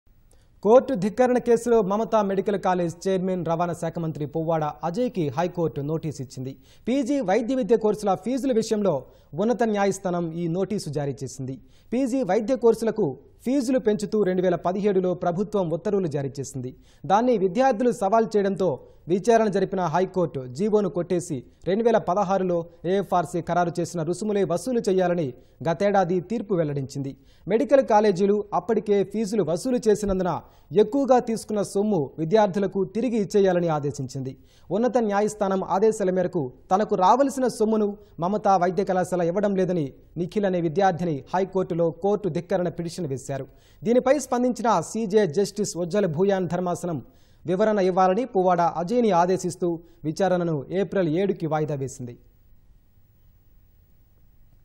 HC Issues Notice to Mamta Medical College Chairman & Minister Puvvada Ajay In Contempt Court Case
కోర్టు ధిక్కరణ కేసులో.. మమత మెడికల్ కాలేజీ ఛైర్మన్, రవాణ శాఖ మంత్రి పువ్వాడ అజయ్ కి హైకోర్టు నోటీసు ఇచ్చింది. పీజీ వైద్య విద్య కోర్సుల ఫీజుల విషయంలో ఉన్నత న్యాయస్థానం ఈ నోటీసు జారీ చేసింది. పీజీ వైద్య కోర్సులకు ఫీజులు పెంచుతూ 2017లో ప్రభుత్వం ఉత్తర్వులు జారీ చేసింది. దాన్ని విద్యార్థులు సవాల్  చేయడంతో విచారణ జరిపిన హైకోర్టు జీవోను కొట్టివేసి.. 2016లో AFRC ఖరారు చేసిన రుసుములే వసూలు చేయాలని గతేడాది తీర్పు వెల్లడించింది. మెడికల్ కాలేజీలు అప్పటికే ఫీజులు వసూలు చేసినందున.. ఎక్కువగా తీసుకొన్న సొమ్ము.. విద్యార్థులకు తిరిగి ఇచ్చేయాలని ఆదేశించింది. ఉన్నత న్యాయస్థానం ఆదేశాల మేరకు తనకు రావల్సిన సొమ్మును మమత వైద్య కళాశాల ఇవ్వడం లేదని నిఖిల్ అనే విద్యార్థిని.. హైకోర్టులో కోర్టు ధిక్కరణ పిటిషన్  వేశారు. దీనిపై స్పందించిన సీజే జస్టిస్ ఉజ్జల్ భూయాన్ ధర్మాసనం.. వివరణ ఇవ్వాలని పువ్వాడ అజయ్ ని ఆదేశిస్తూ విచారణను ఏప్రిల్ 7కి వాయిదా వేసింది